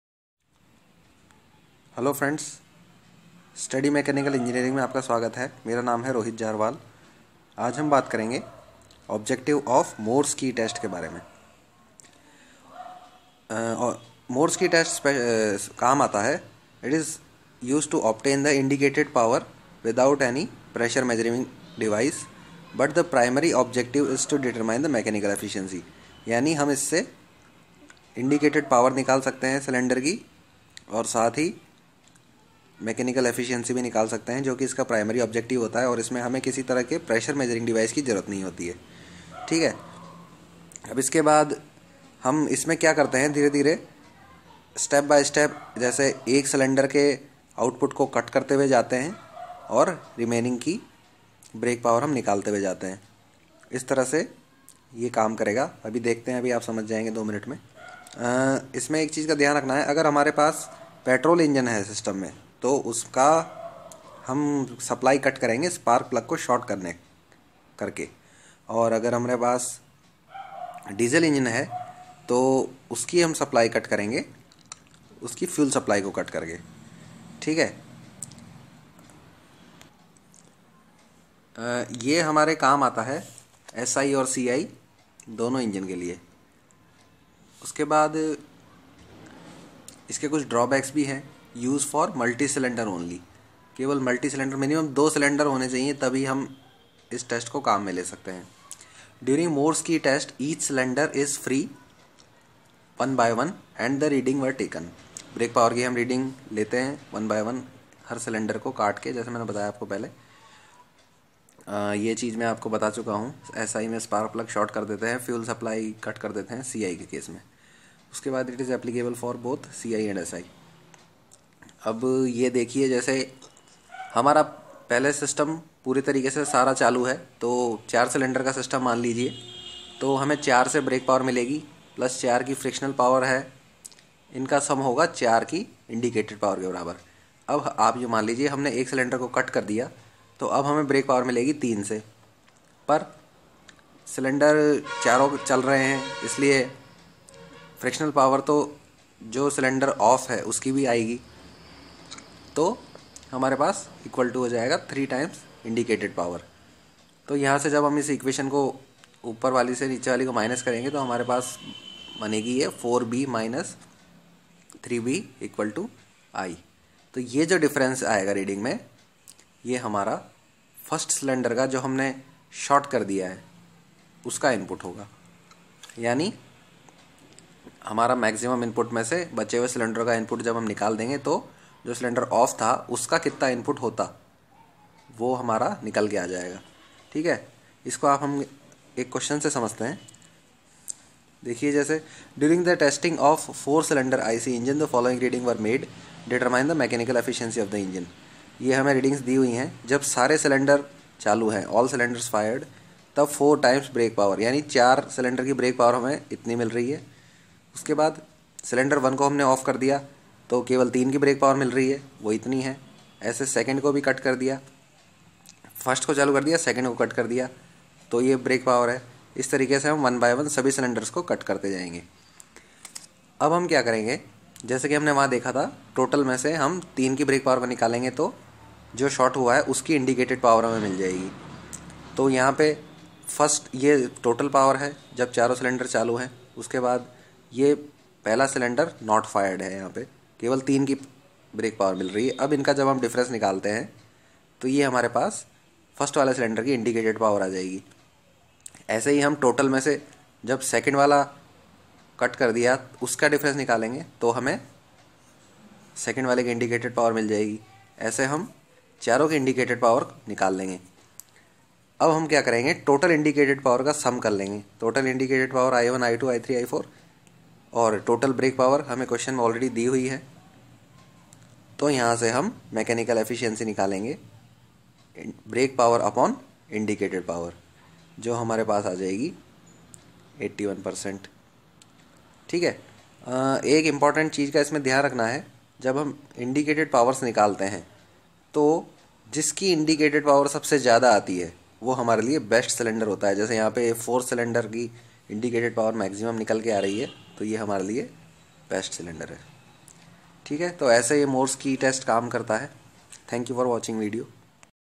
MORSE TEST by study mechanical engineering
In this video you will learn about how morse test is carried out.  It will help you to perform experiment in your lab.